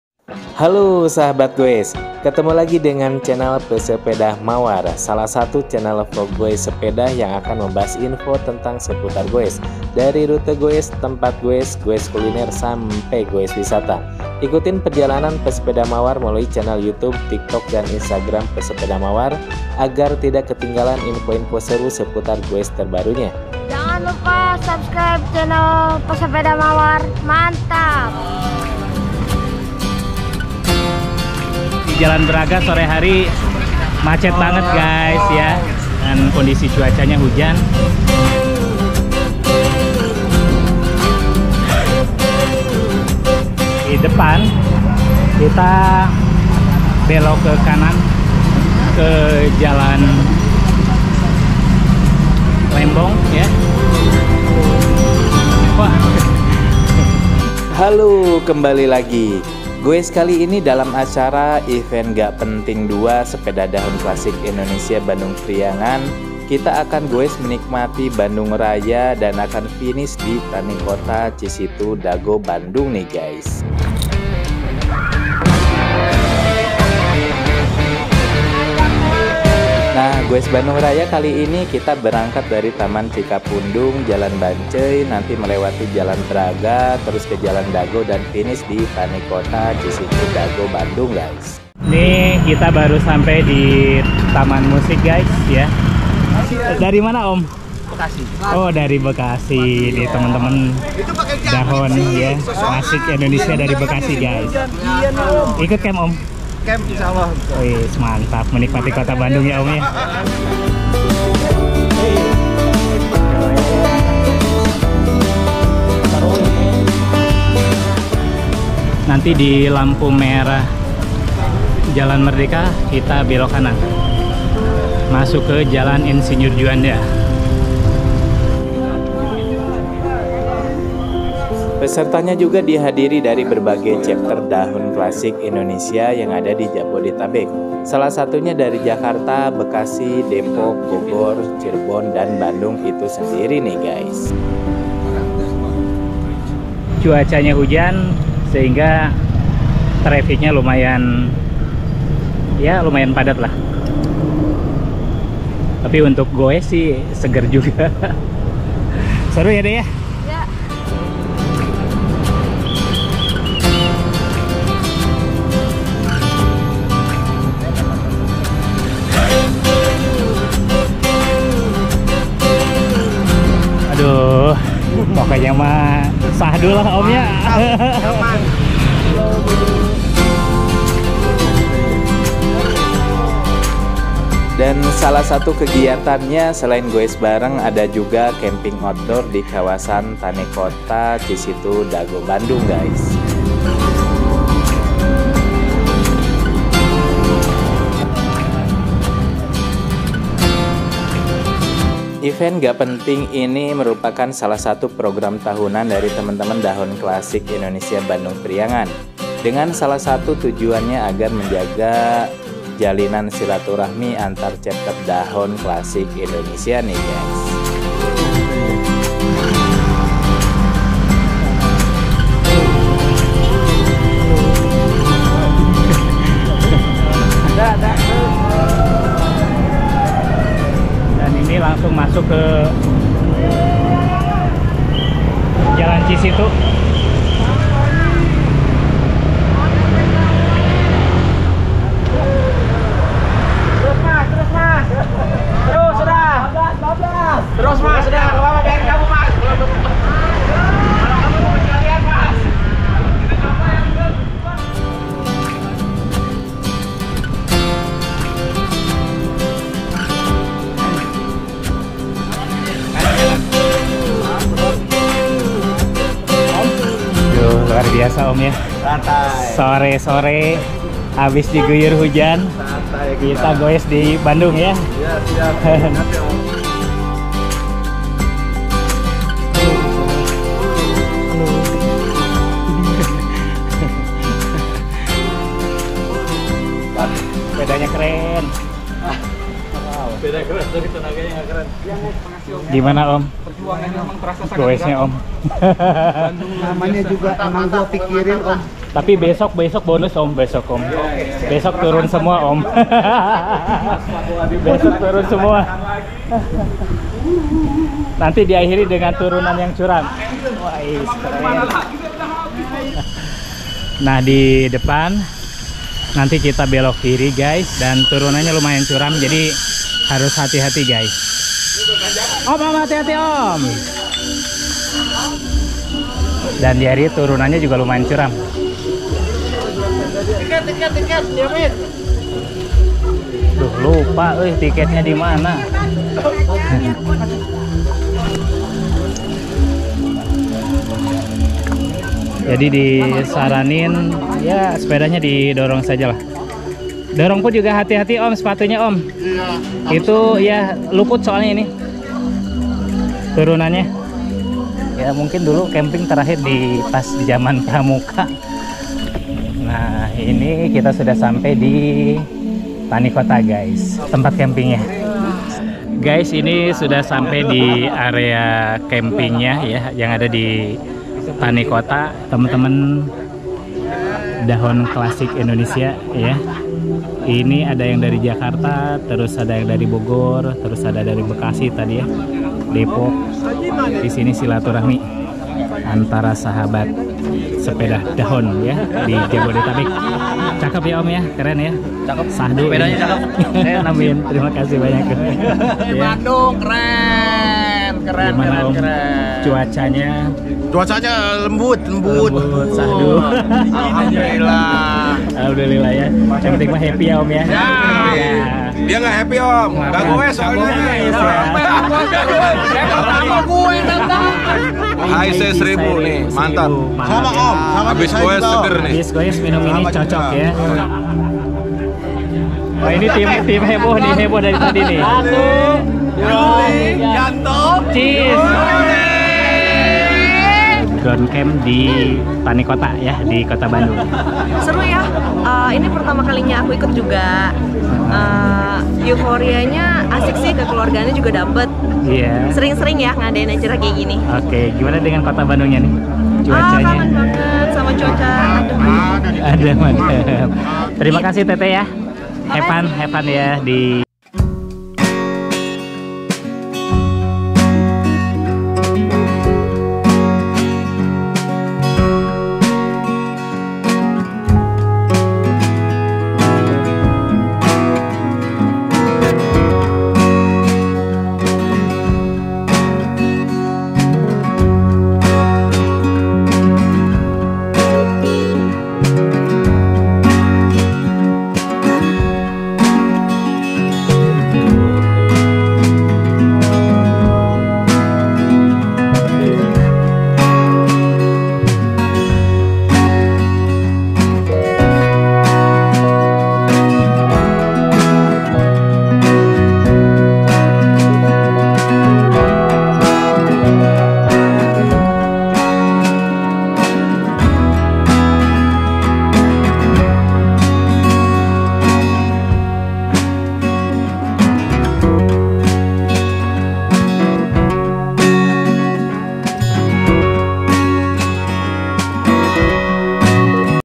GOWES BANDUNG RAYA SEPEDA DAHON CLASIC INDONESIA DALAM ACARA EVENT GA PENTING 2022 - GOWES BANDUNG
Gowes Bandung Raya sepeda dahon clasic Indonesia kali ini dalam acara memeriahkan Event Tahunan Komunitas Sepeda Dahon Clasic Indonesia Bandung Priangan yang di beri nama EGP 2 atau Event Ga Penting 2 ini di hadiri oleh berbagai chapter komunitas sepeda Dahon clasic Indonesia di Jabodetabek, seperti dari komunitas Sepeda Dahon Clasic Jakarta, Bekasi, Depok, Bogor, Cirebon,serta dari  Bandung itu sendiri.

Terimakasih

Salam

Pesepeda Mawar